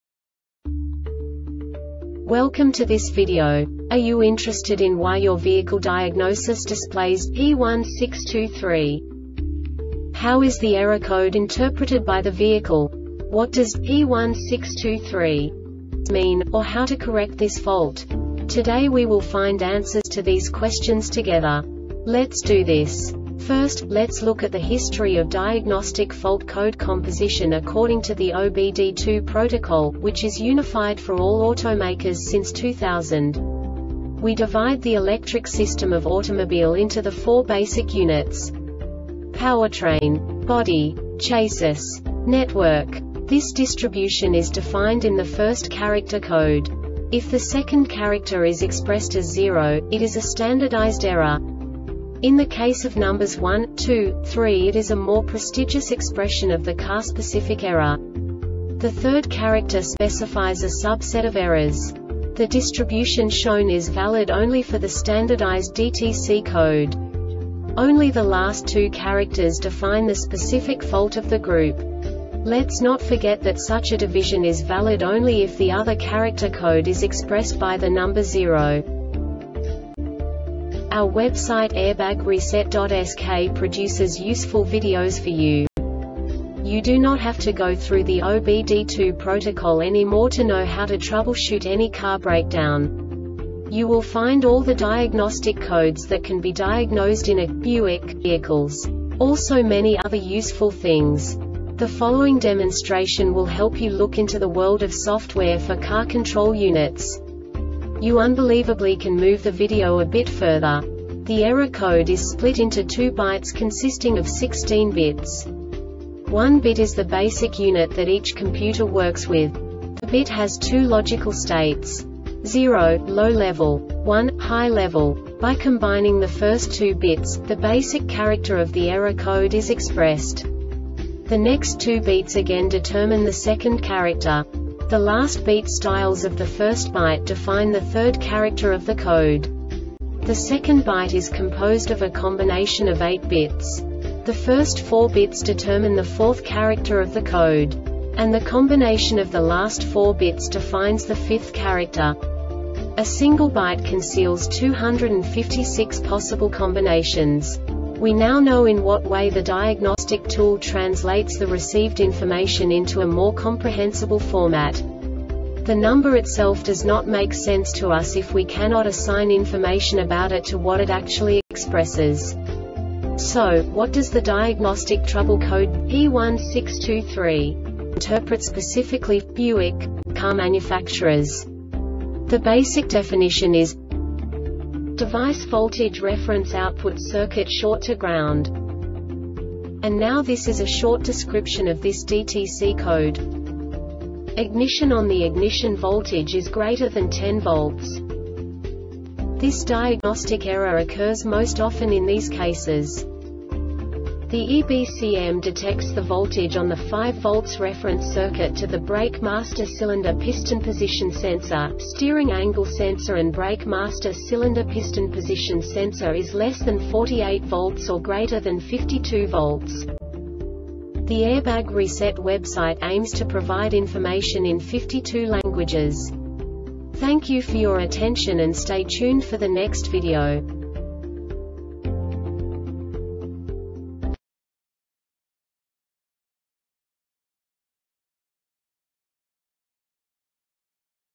DTC Buick P1623 Short Explanation
Contents:
0:21 Basic DTC analysis according to OBD2 protocol standard.
1:48 Insight into programming
2:58 A diagnostically understandable description of the Diagnostic Trouble Code
3:05 Basic error definition

Make:
Buick
Code:
P1623
Definition:
Device Voltage Reference Output Circuit Short to Ground
Description:
Cause:
The EBCM detects the voltage on the 5 V reference circuit to the brake master cylinder piston position sensor,steering angle sensor and brake master cylinder piston position sensor is less than 4.8 V or greater than 5.2 V.